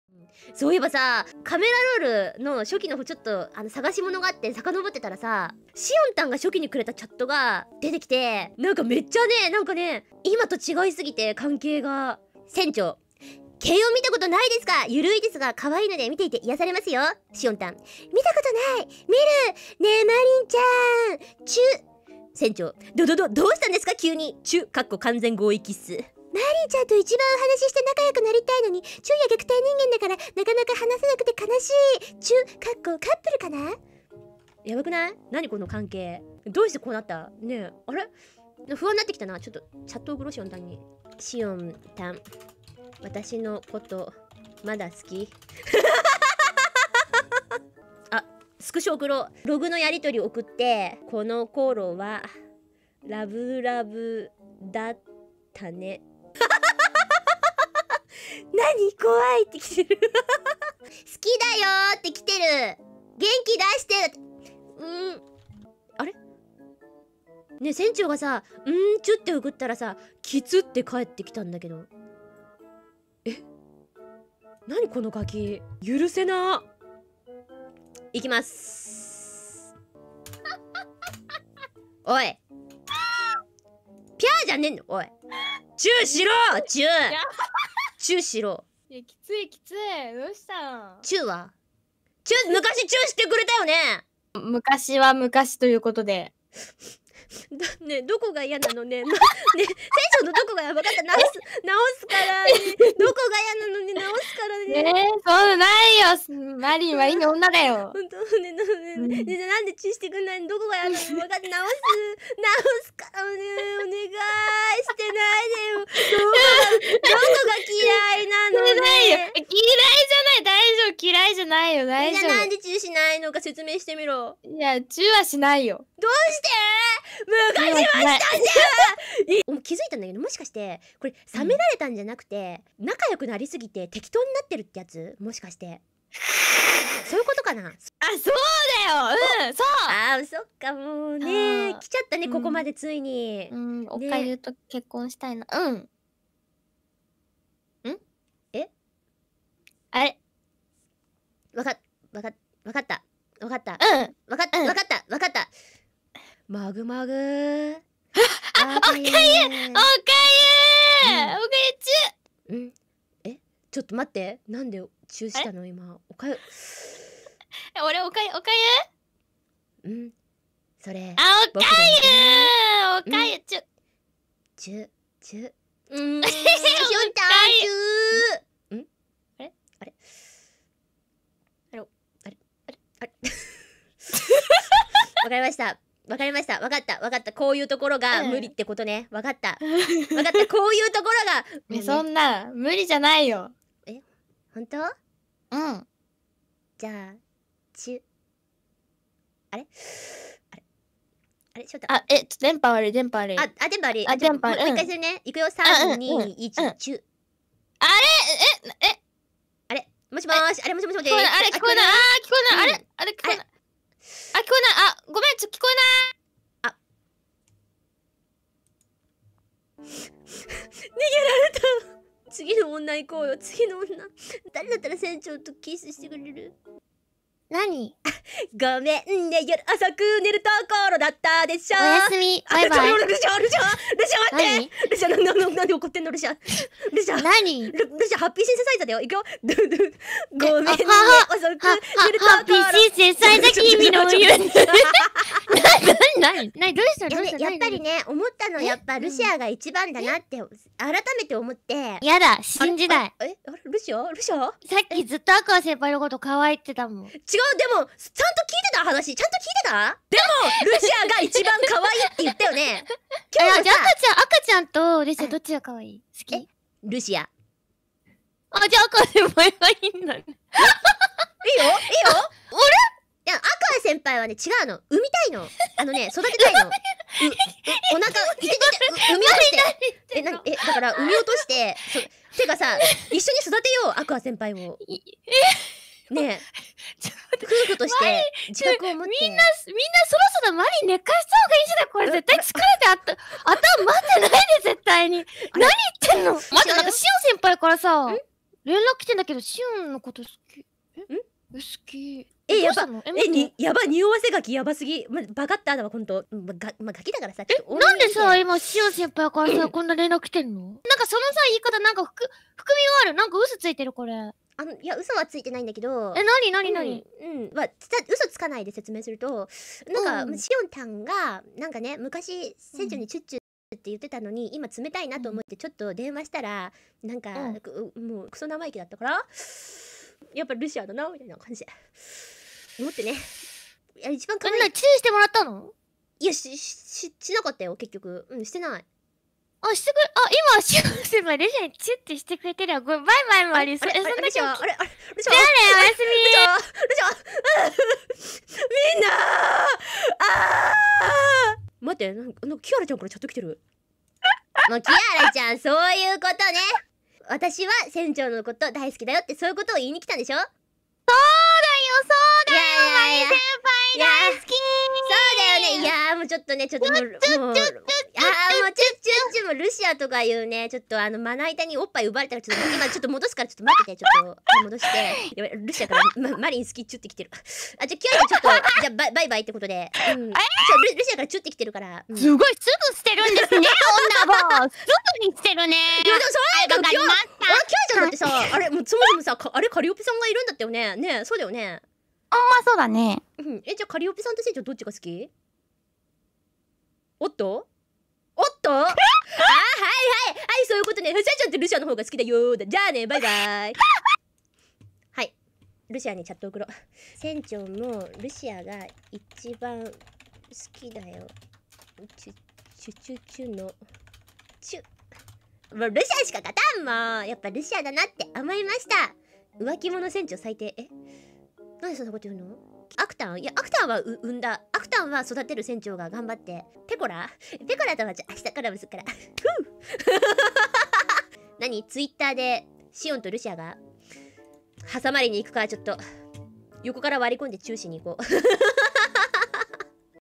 深夜1時、都合のいい女を漁りだすめんどくせー女マリン【ホロライブ】
負けヒロインの魂がこびりついてる

8:51:22 pm